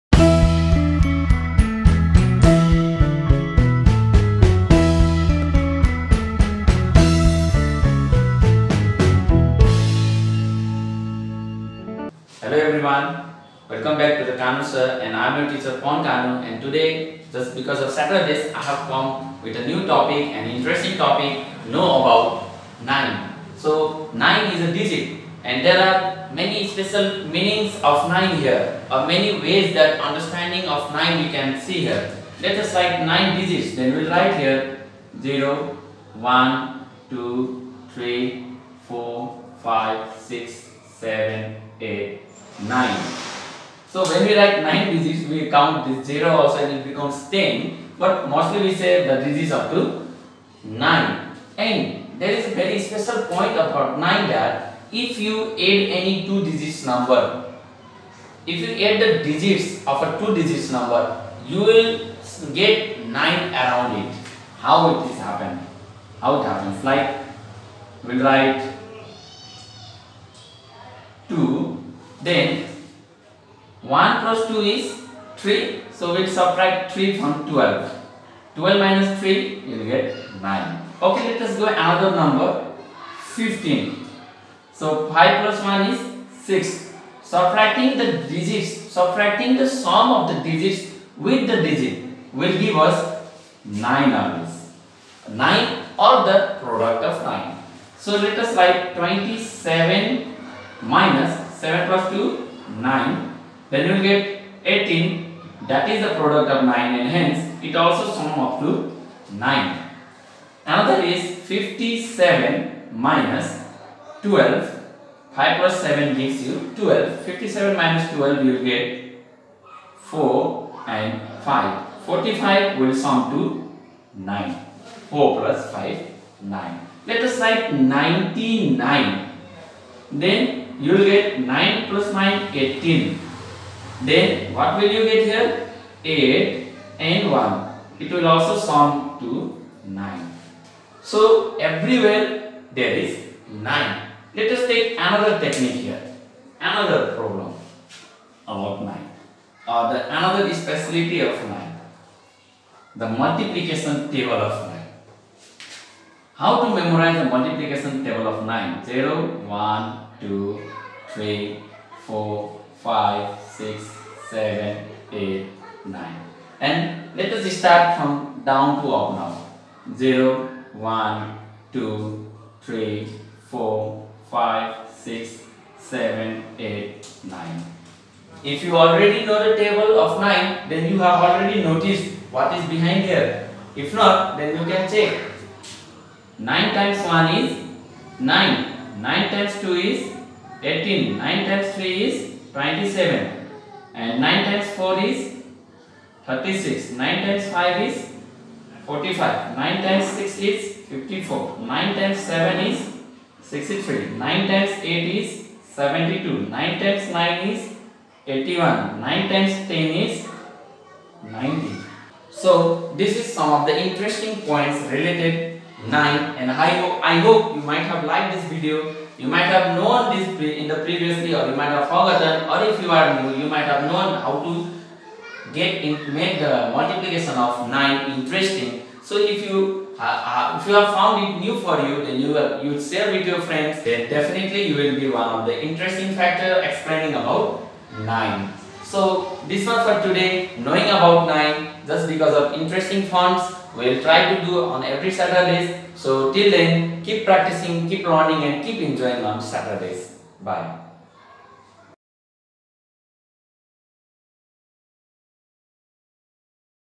Weekend special: Is 9 really Mysterious???? l Pawan kanu
Here I have explained about the special conditions by which number nine appears its self during the calculatiins.